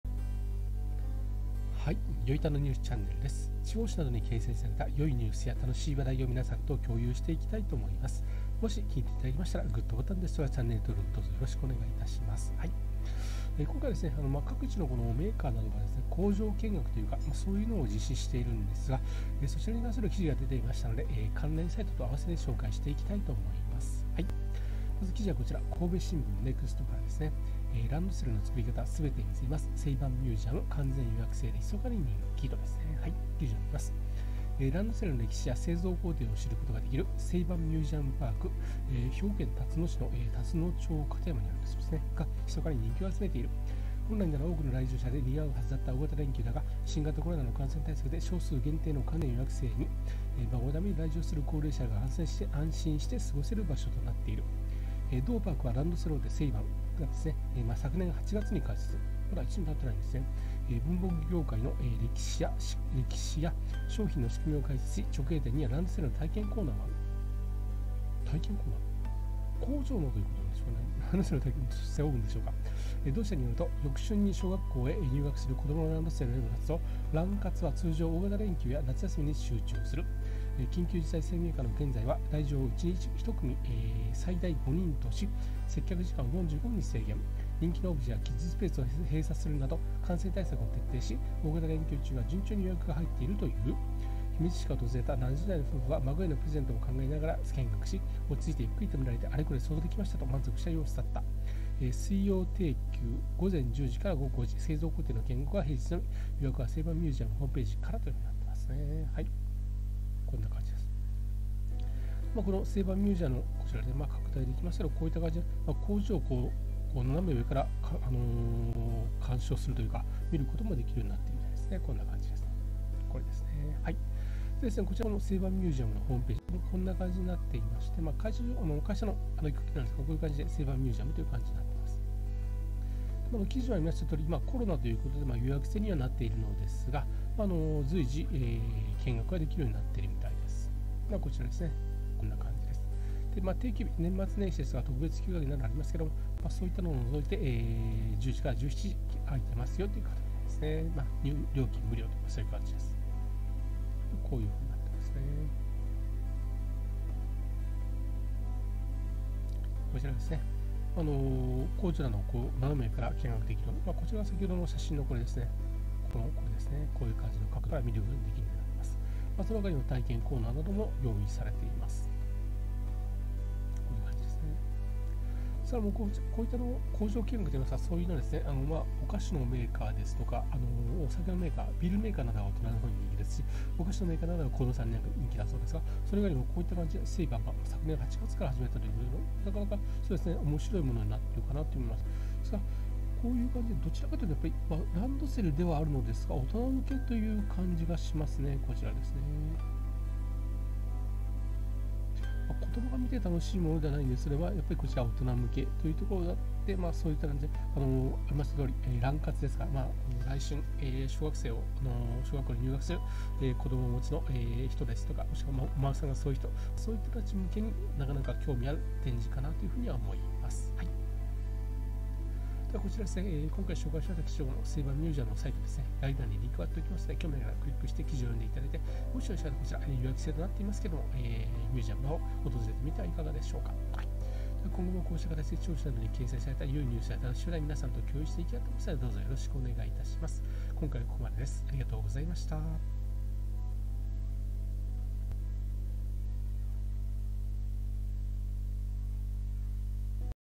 【良楽ニュース】ランドセルの作り方って知っていますか？
ランドセルメーカーの「セイバンミュージアムパーク」が人気だそうです。

【紹介記事】
神戸新聞「ランドセルの作り方、すべて見せます　セイバンミュージアム　完全予約制でひそかに人気」

記事や写真など全ての権利は著作者が保有しております。法律における引用の目的で使用しておりますが、問題がありましたらご連絡ください。よろしくお願いいたします。